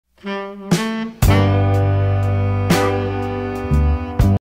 First animation
A 4 second clip that took me half an hour to make. (Yes I know the lip sync is terrible)